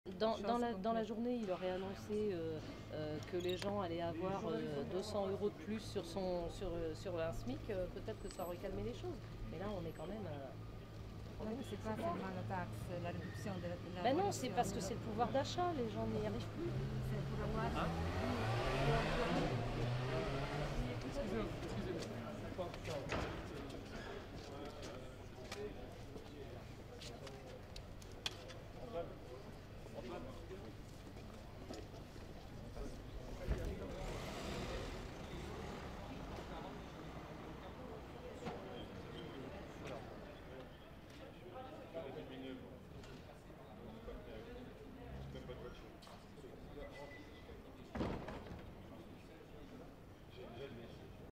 Une délégation de "gilets jaunes" arrive à Matignon
Le Premier ministre Edouard Philippe reçoit une délégation de six "gilets jaunes libres", à la veille de l'acte IV du mouvement de protestation. La délégation du collectif, qui a appelé les "gilets jaunes" à renoncer à manifester samedi à Paris, sera notamment composée de Benjamin Cauchy, de Jacline Mouraud et de Cédric Guémy.